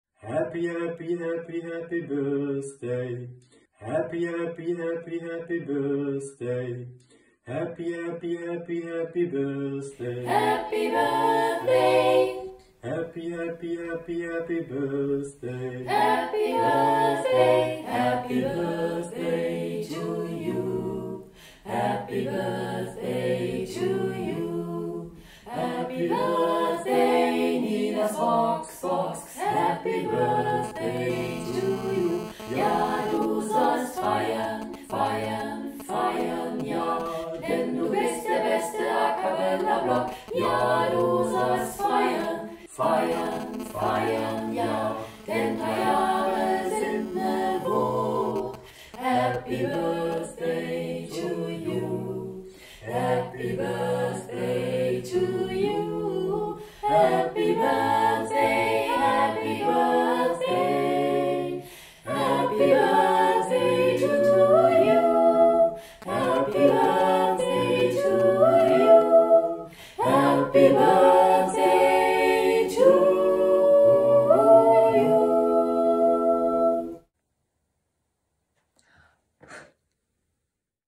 chor:werk baden-württemberg gratuliert zum 3. Geburtstag von Ninas VoxBox
Vielen lieben Dank an das Chor:werk baden-württemberg für die Grüße zum 3. Geburtstag von Ninas VoxBox 🥳🎉🎶 Wenn uns Corona derzeit nicht kulturell blockieren würde, wäre der Chor mit seinem neuen Kinderprogramm ähhhcapella diesen Monat bei mir im Humboldtsaal Freiburg aufgetreten. 🙁